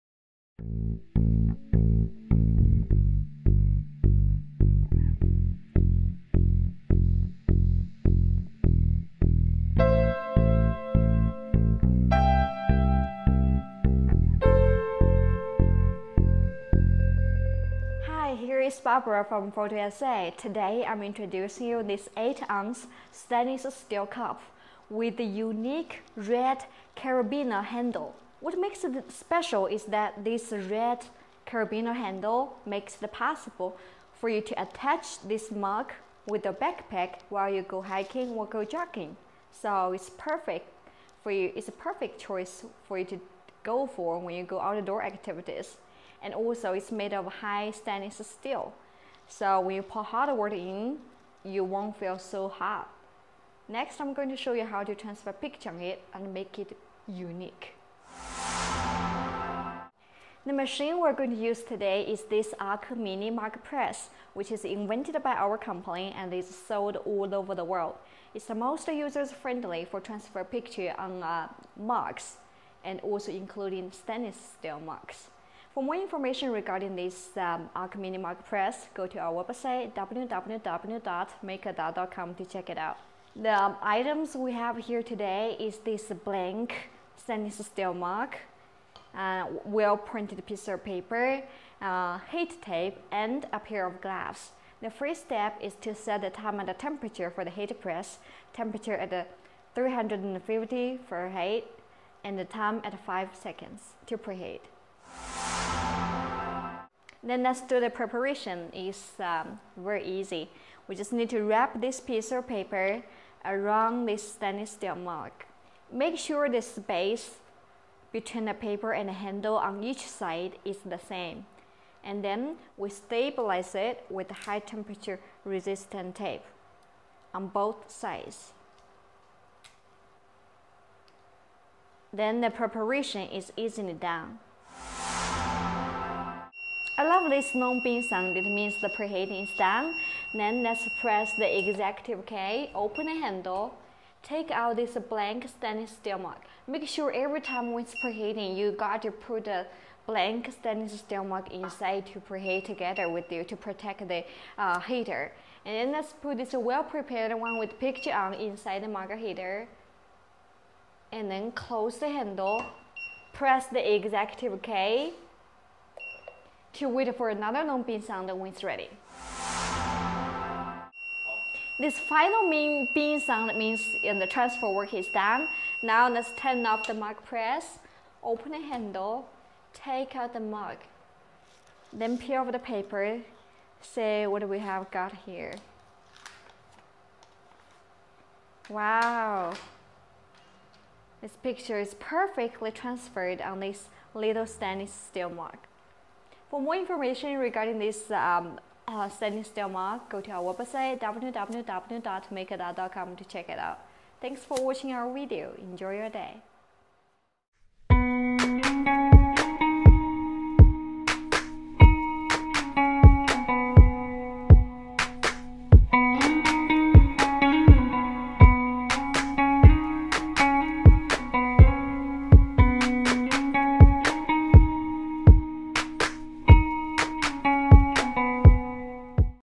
Stainless Steel Mug w/ Red Carabineer Handle - Photo USA
In this video, we'll show you excetly how to sublimate this functional stainless steel mug. All the sublimation skills will be explained by Photo USA.